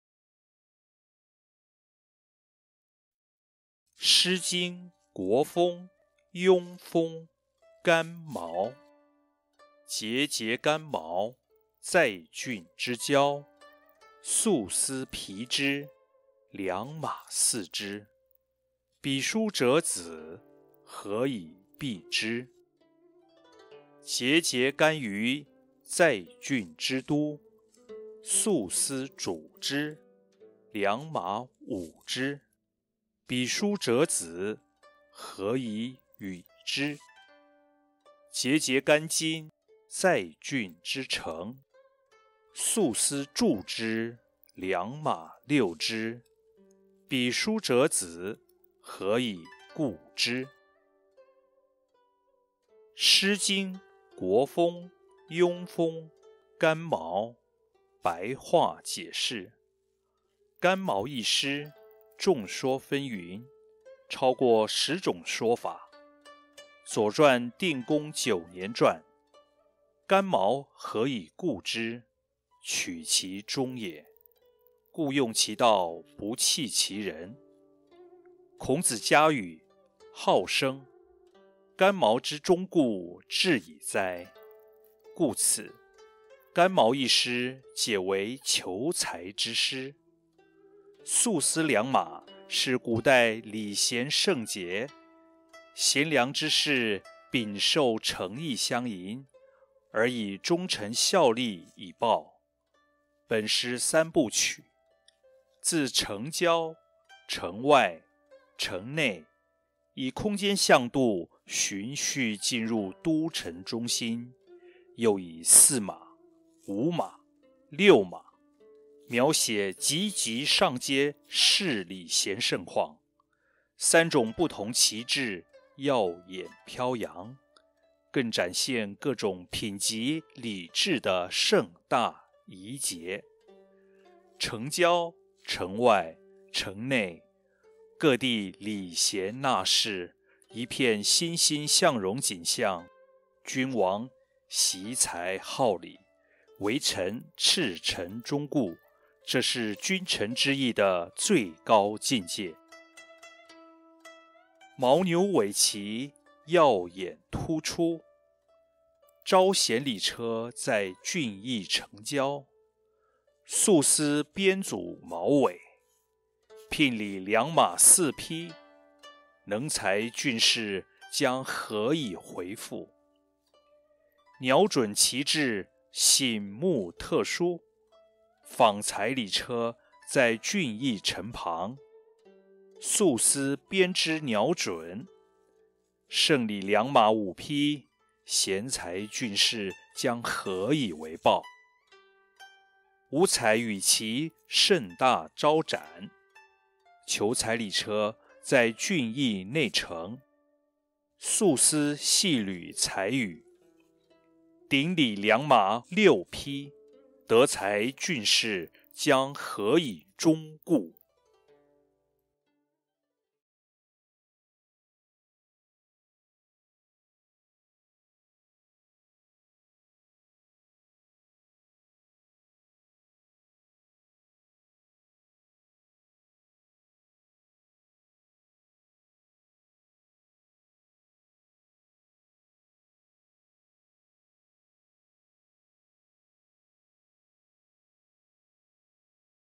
詩經有聲書｜國風｜鄘風｜干旄 🔹風簷細品詩經🔹
🔹詩經有聲書🔹
詩經｜國風｜鄘風｜干旄
曉聞達張主筆｜曹興亞輔正｜王狄秀影音編輯

■ 詩文

孑孑干旄，在浚之郊。
素絲紕之，良馬四之。
彼姝者子，何以畀之？

孑孑干旟，在浚之都。
素絲組之，良馬五之。
彼姝者子，何以予之？

孑孑干旌，在浚之城。
素絲祝之，良馬六之。
彼姝者子，何以吿之？

■ 導言釋義

干旄一詩，眾說紛紜，超過十種說法。《左傳‧定公九年傳》干旄何以告之，取其忠也，故用其道不棄其人。《孔子家语·好生》干旄之忠告至矣哉。故此，干旄一詩解為求才之詩。素絲良馬是古代禮賢盛節。賢良之士稟受誠意相迎，而以忠誠效力以報。本詩三部曲。自城郊，城外，城內。以空間向度循序進入都城中心。又以四馬，五馬，六馬，描寫級級上階是禮賢盛况。

三種不同旗幟耀眼飄揚，更展現各種品級禮制的盛大儀節。城郊，城外，城內。各地禮賢納士，一片欣欣向榮景象。君王惜才好禮，為臣赤誠忠告。這是君臣之義的最高境界。

■ 白話釋文

旄牛尾旗耀眼突出，召賢禮車在浚邑城郊，素絲編組旄尾，
聘禮良馬四匹，能才俊士將何以回覆。

鳥隼旗幟醒目特殊，訪才禮車在浚邑城旁，素絲編織鳥隼，
盛禮良馬五匹，賢才俊士將何以為報。

五彩羽旗盛大招展，求才禮車在浚邑內城，素絲細縷彩羽，
頂禮良馬六匹，德才俊士將何以忠告!